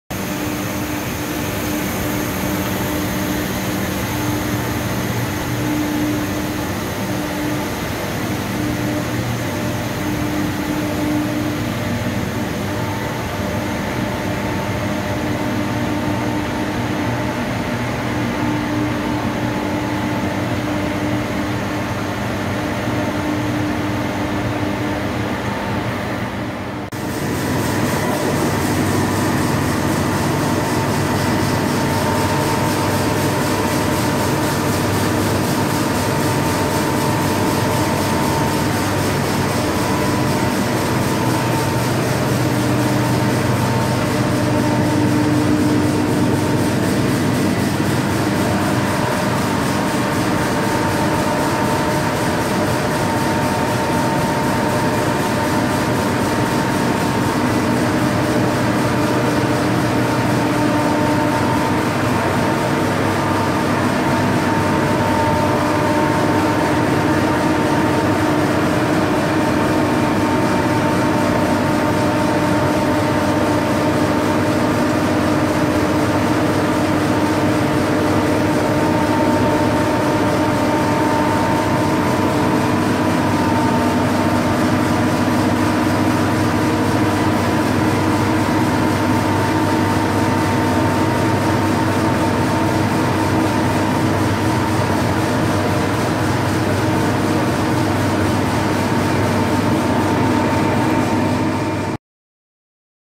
USED CMG FILM EXTRUDER 70 mm screw 1700 mm
USED FILM EXTRUDER 70 mm screw, die diam 240 mm. corona treatment, 1700 mm single collection for tube,  slit and seal units to get 3 reels. Good overall conditions. running